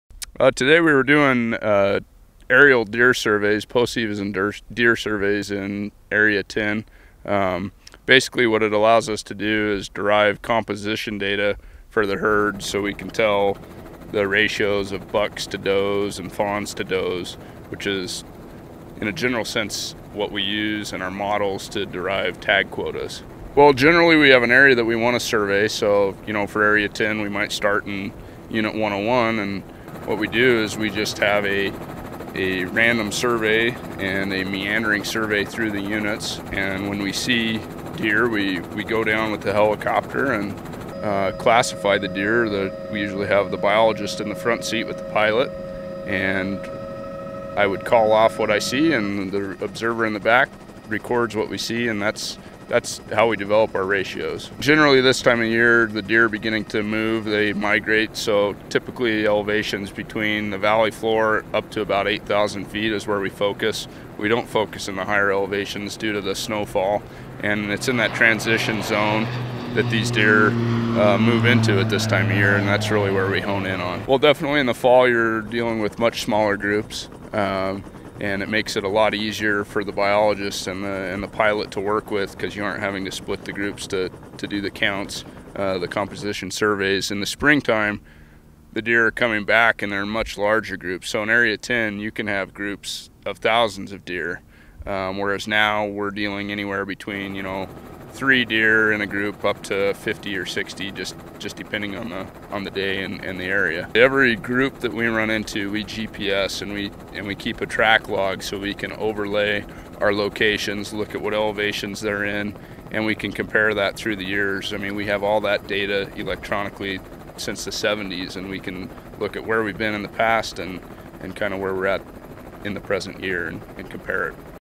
A Look at How NDOW Biologists Count Deer
Ever wonder how Nevada Department of Wildlife biologists count deer?  On this Wildlife Wednesday,  NDOW Biologist Caleb McAdoo explains how it’s done.